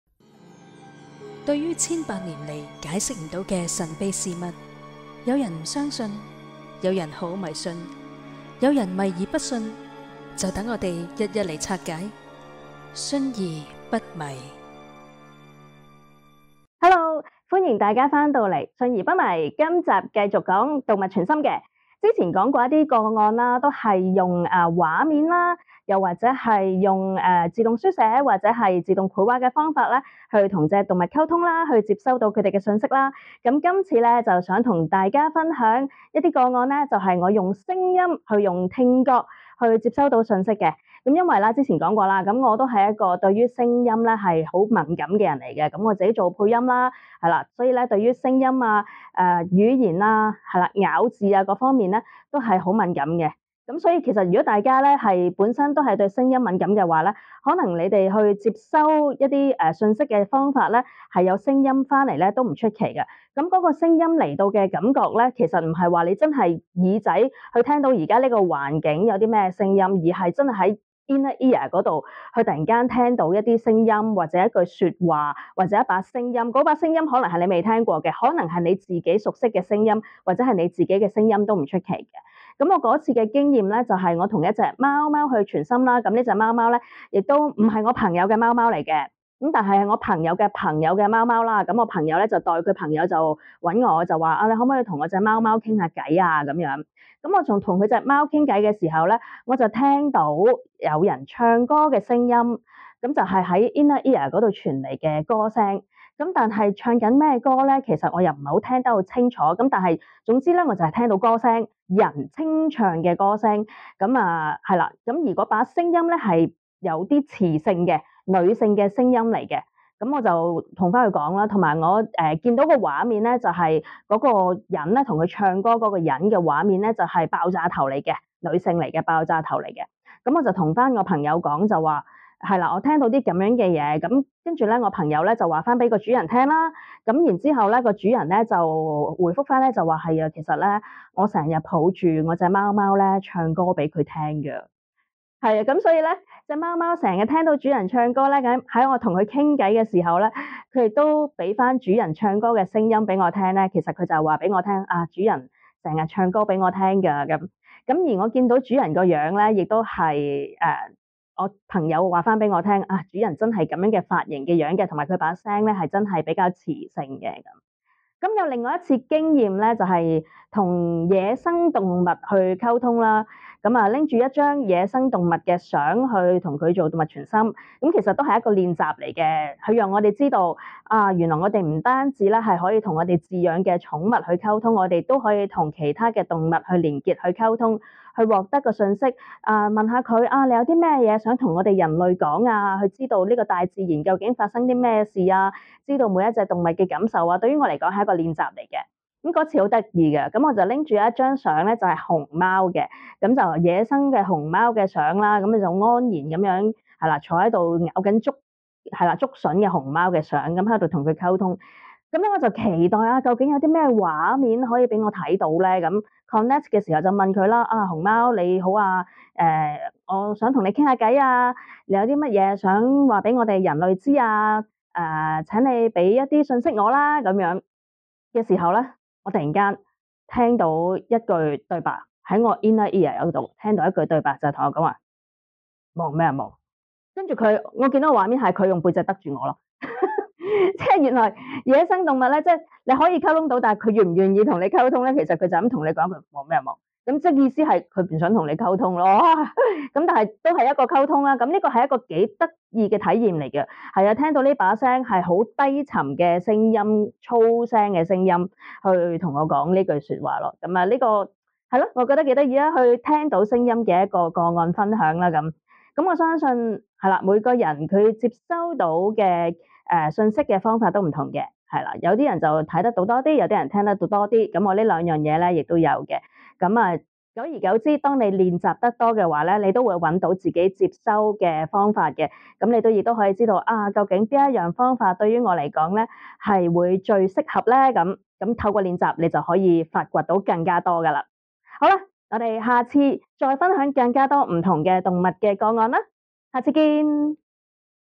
【信而不迷】動物傳心個案分享：透過聽覺接收訊息，貓貓分享主人唱歌的聲音
跟動物進行傳心時，我們除了能接收到畫面外，也可能會聽到聲音。這次要分享的個案是跟貓貓傳心時，很清楚地聽到主人的歌聲。

有關動物傳心課程的詳情可到以下平台瀏覽。